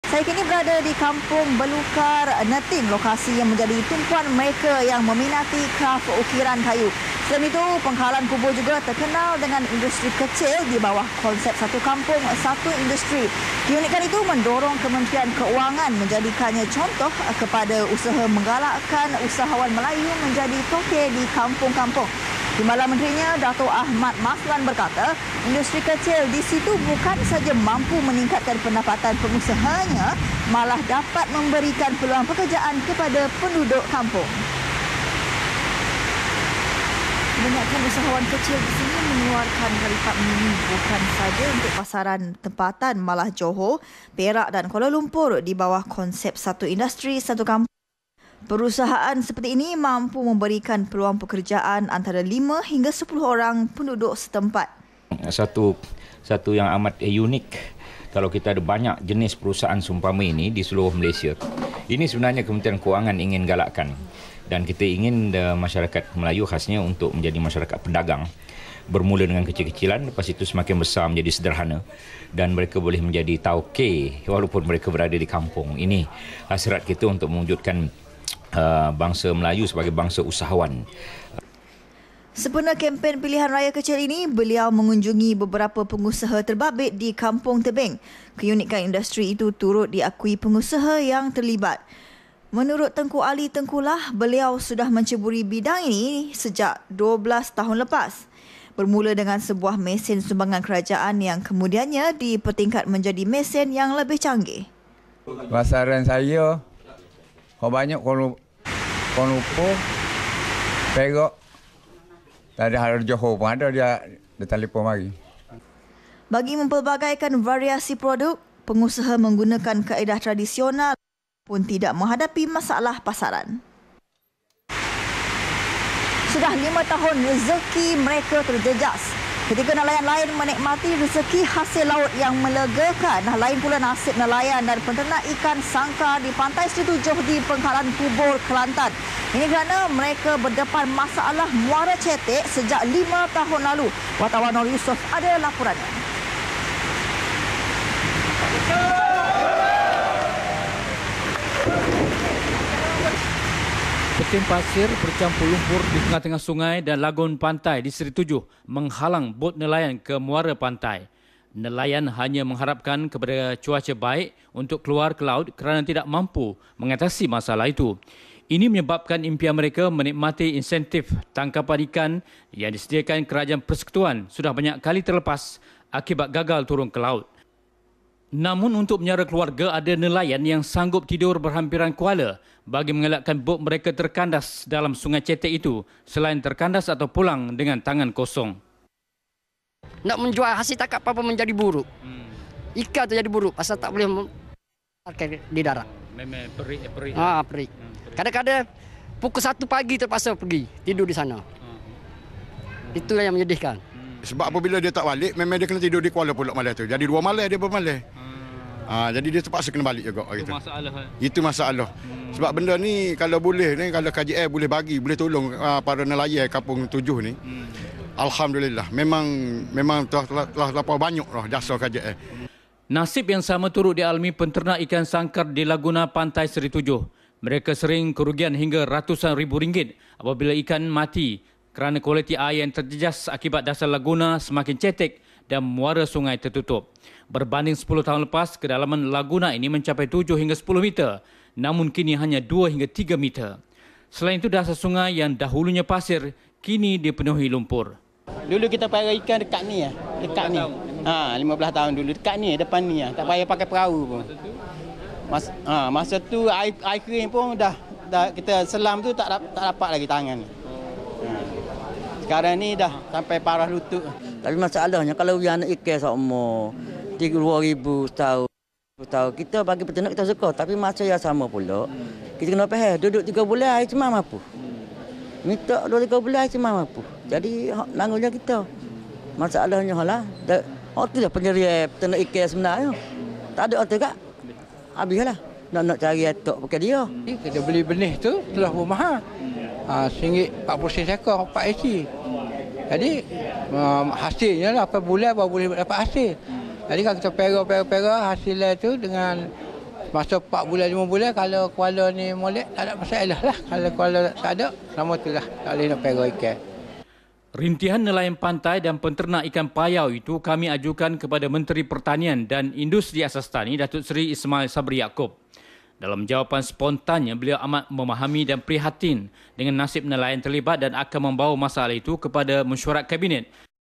LAPORAN PRK N.01 - 5 PM [17 Sept 2014]
Keunikan industri kecil di Pengkalan Kubor akan dijadikan contoh kepada usaha membangunkan usahawan kecil di kawasan lain. Ia unik bukan sahaja kerana  pengusaha berjaya meningkatkan pendapatan malah mampu memberikan peluang pekerjaan kepada penduduk kampung.

Ikuti Laporan sesi petang  bersama Delilah Kadir  dari Kampung Belukar Neting, Pengkalan Kubor, Kelantan.